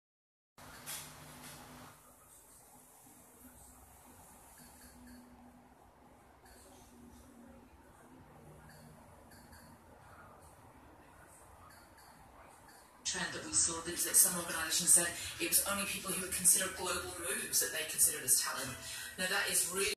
Home Automation - Radio On
With your smart phone, you can turn on the radio with just a touch.